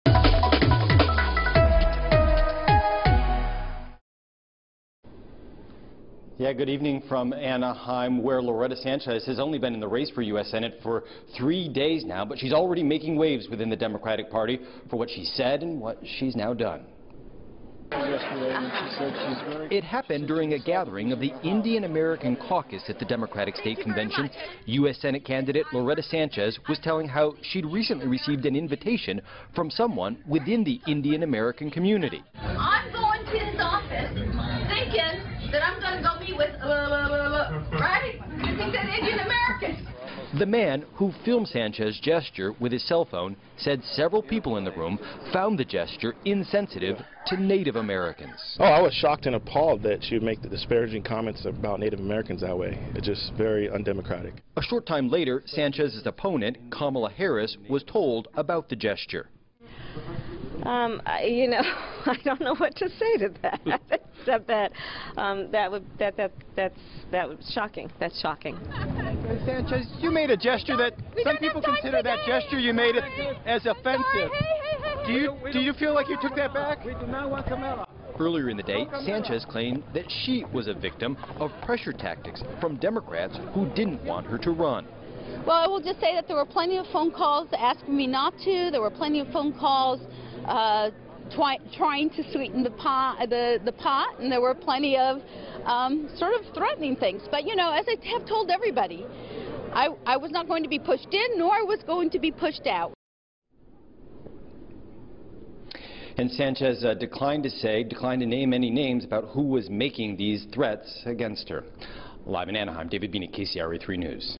Congresswoman dodges questions about Native American remark, gesture
She's only been in the race for three days now, but Congresswoman Loretta Sanchez is making waves for what she said and what she has now done.